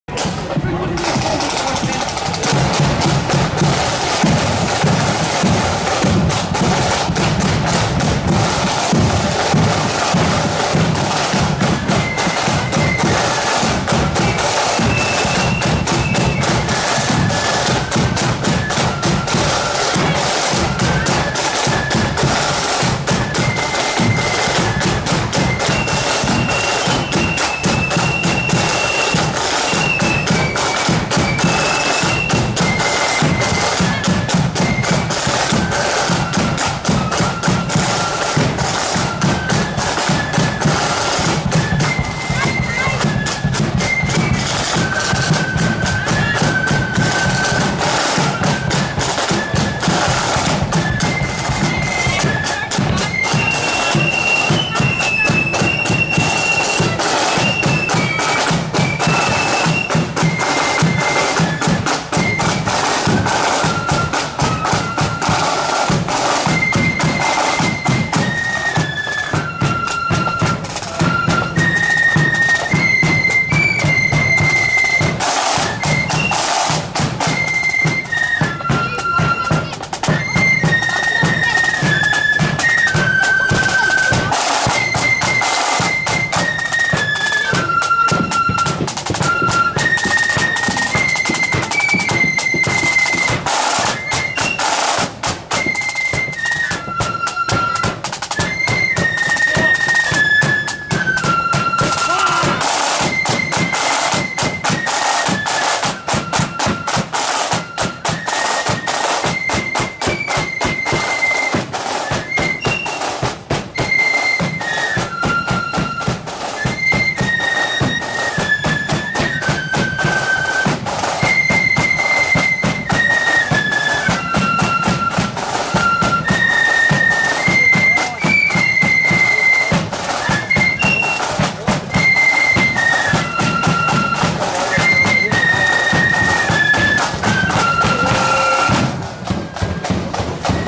Drumderg Loyalists (No.3) @ Loyal Sons Of Benagh's Parade ~ 21/04/23 (4K)
Drumderg Loyalists Flute Band parading through the streets of Newry at Loyal Sons Of Benagh Flute Band's annual parade held on Friday 21/04/2023.